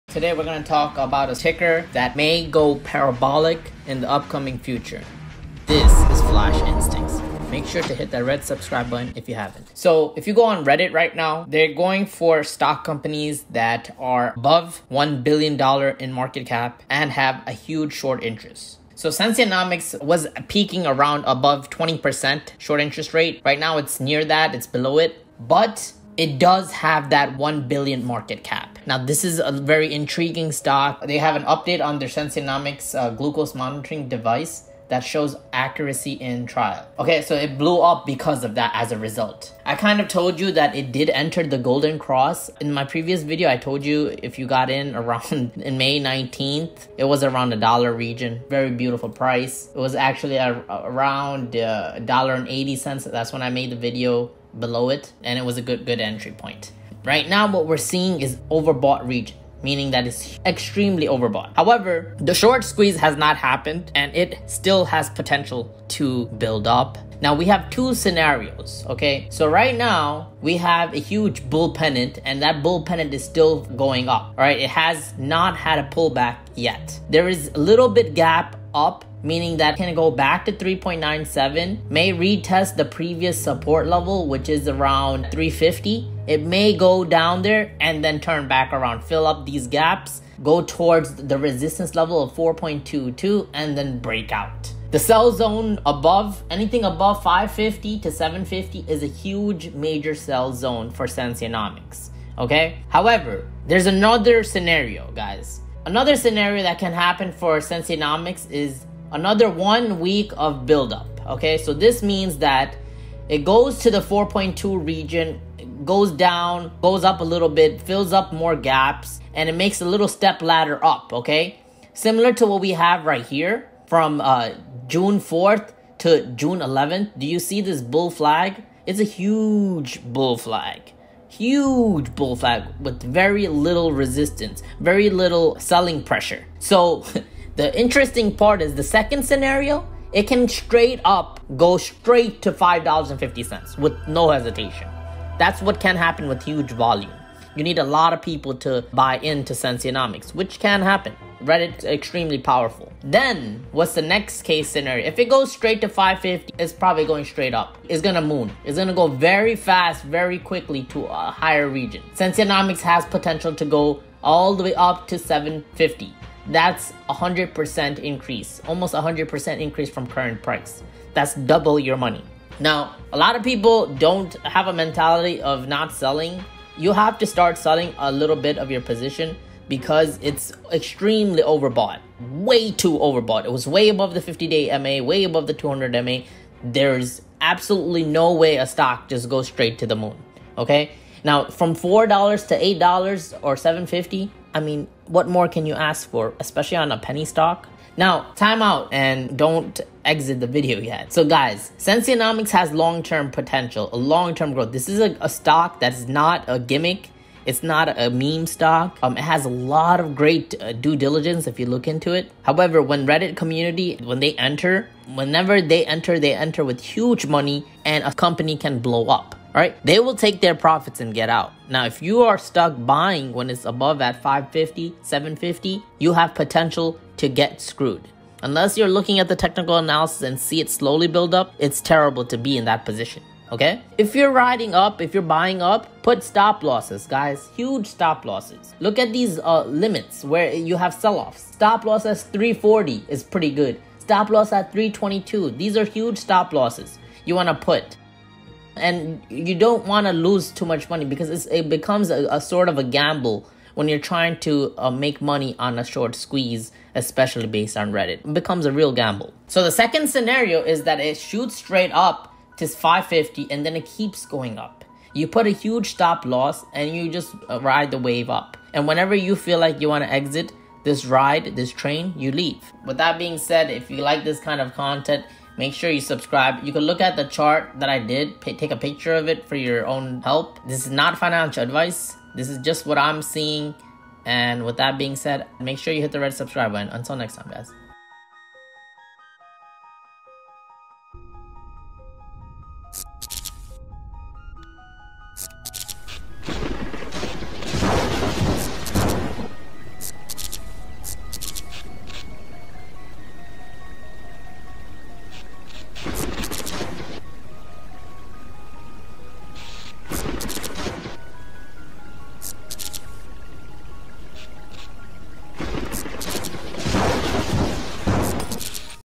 SENS short squeeze potential & how to react to it [TA]
we look at sens, and perhaps shortsqueeze, reddit situation.

Flash Instincts provides insight in a UNIQUE and Different way to stocks based on chart patterns and DD.